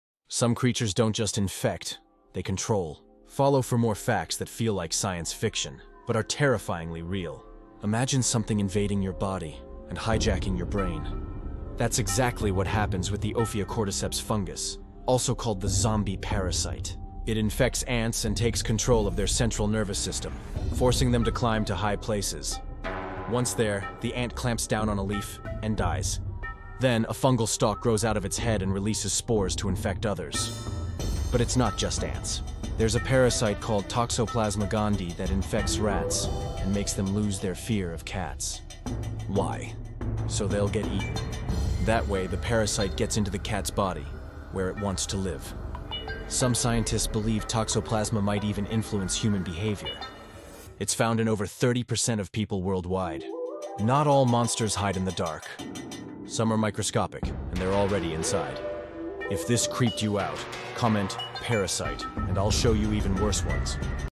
This Parasite Controls Its Victims Like a Puppet 🧠🪱 | Creepiest Nature Fact
Some parasites don’t just infect — they take over.
From zombie ants to fearless rats, these real-life parasites hijack brains and control behavior.
One even infects over 30% of the human population... without us knowing.